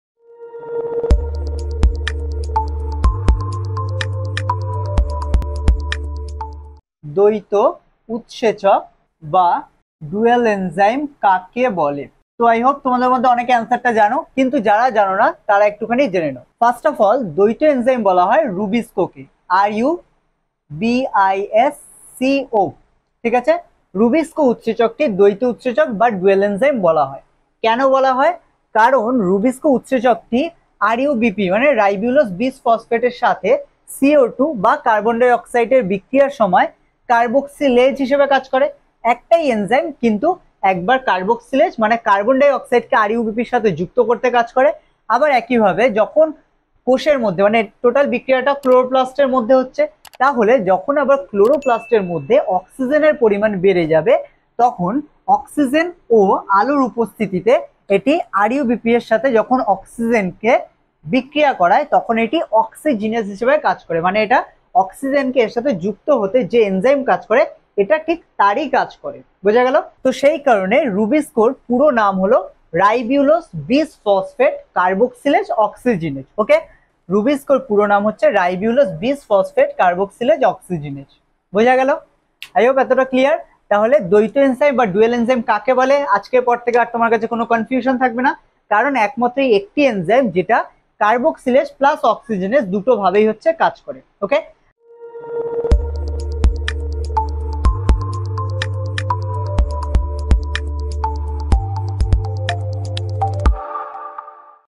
Dual Enzyme | দ্বৈত উৎসেচক | Photosynthesis | Plant Physiology | Biology Terms
প্রিয় বন্ধুরা,
বঙ্গ পাঠশালা(bongo pathshala)য় তোমাদের স্বাগত।। এই ক্লাস এ আমরা  Dual Enzyme নিয়ে আলোচনা করেছি।।

আশাকরি ক্লাস টি তোমাদের ভালো লাগবে, শেষ পর্যন্ত দেখার অনুরোধ রইল।।